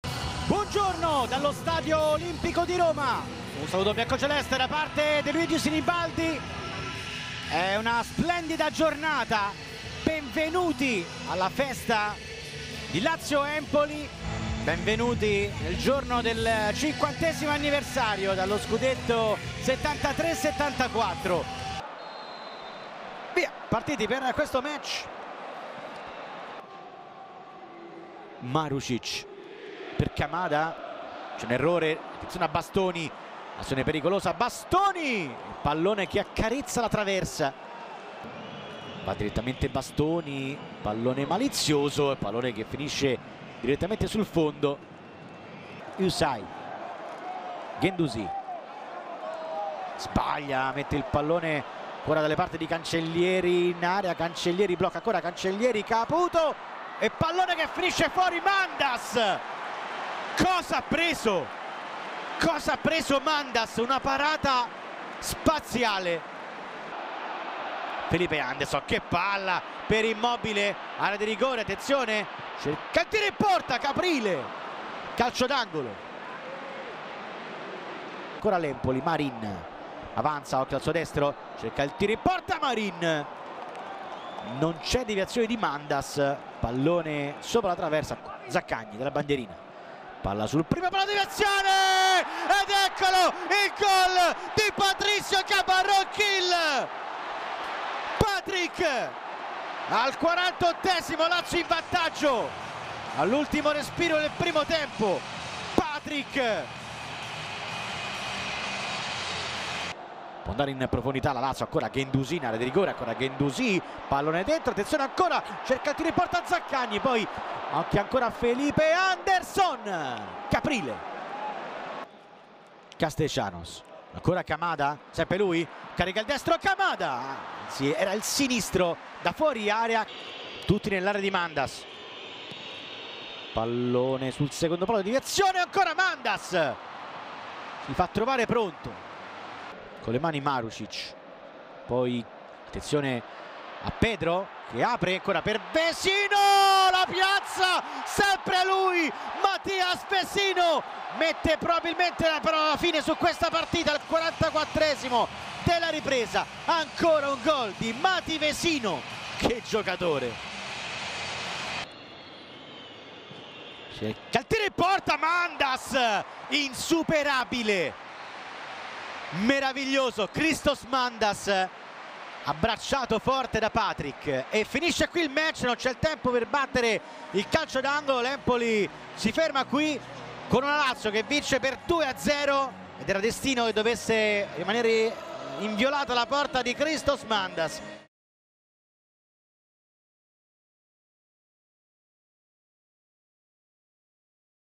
⏪ HIGHLIGHTS | L`ultimo Lazio-Empoli
Riviviamo la vittoria biancoceleste del 12 maggio 2024, arrivata grazie alle reti di Patric e Vecino!